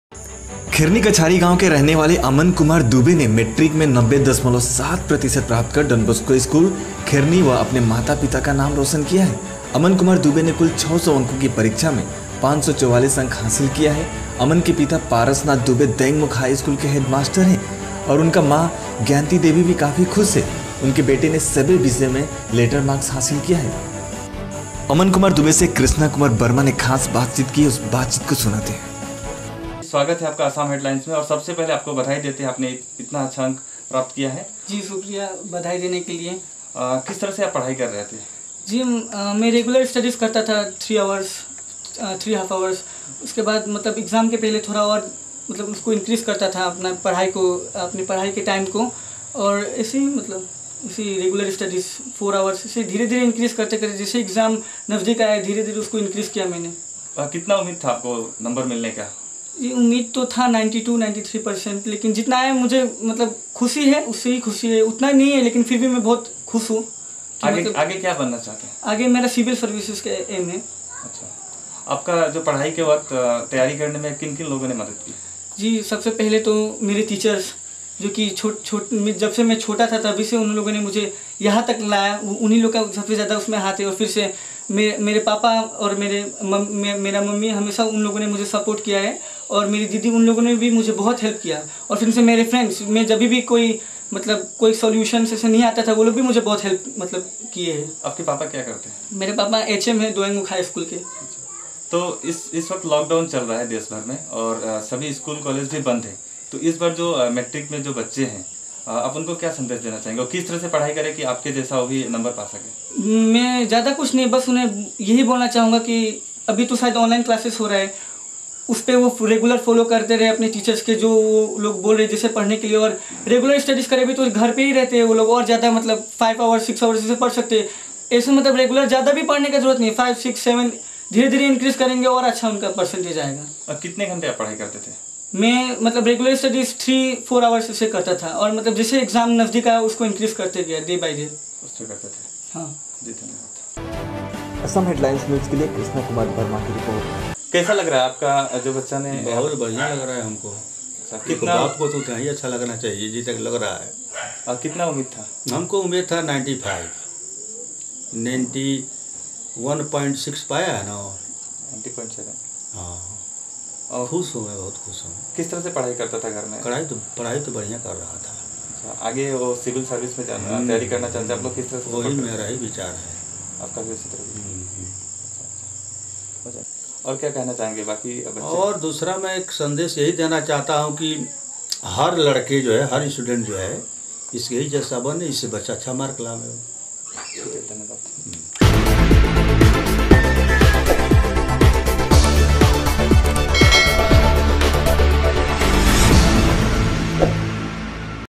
Kheroni गाँव के रहने वाले अमन कुमार दुबे ने मैट्रिक में 90.7 % प्राप्त कर गाँव का नाम रोशन किया है
खेरनी कछारी गाँव के रहने वाले अमन कुमार दुबे ने मैट्रिक में 90.7 प्रतिशत  प्राप्त कर Donbosco स्कूल ,खेरनी ,अपने माता पिता का नाम रोशन  किया है।अमन कुमार दुबे ने कुल 600 अंको की परीक्षा में 544 अंक हासिल किया हैं.अमन के पिता पारसनाथ दुबे दयंग मुख हाई स्कूल के हेडमास्टर है.